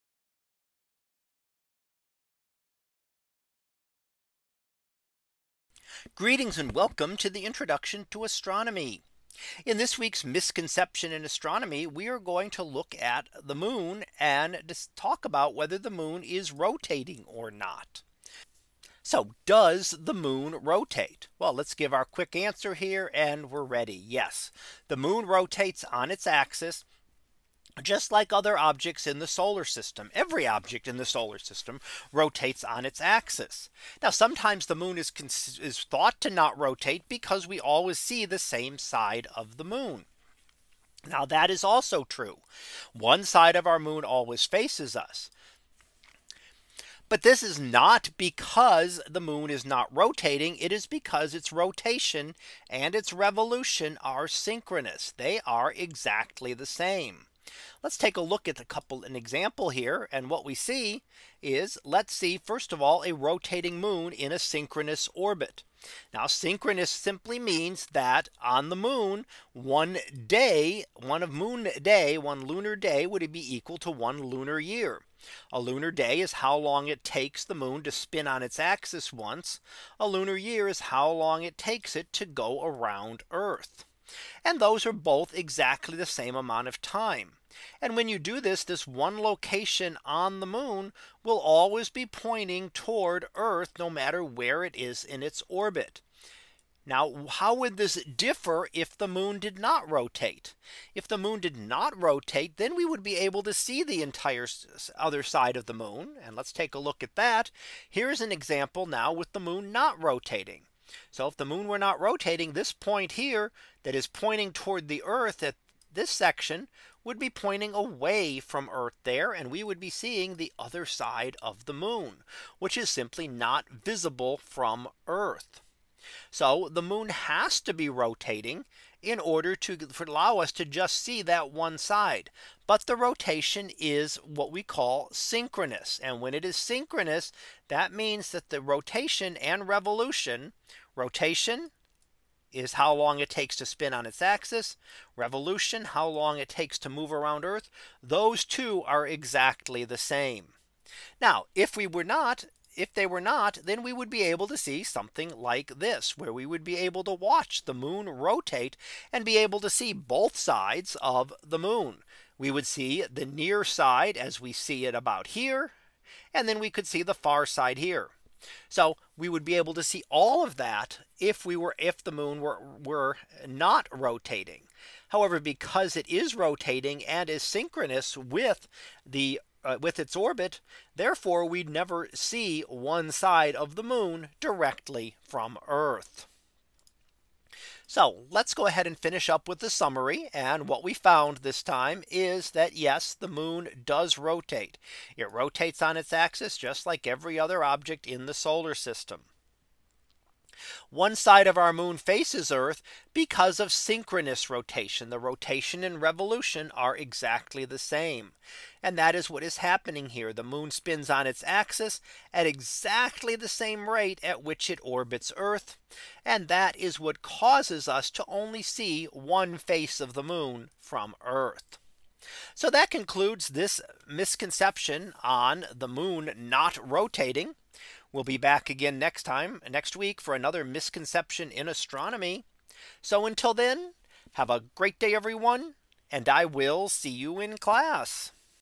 Misconceptions in Astronomy - The Moon is Not Rotating
In this lecture, we will look at the misconception that our Moon is not rotating. While it is correct that only one side of our Moon is visible from Earth, this is because it is rotation in a synchronous manner and not that it does not rotate.